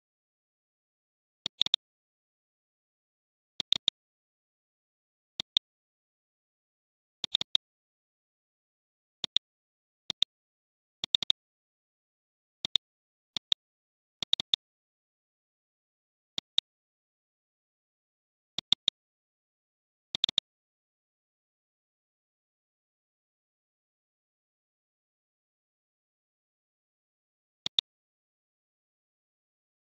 Fusion360 スカルプ おすすめ選択方法
エッジループの選択、面のループの選択、選択範囲の反転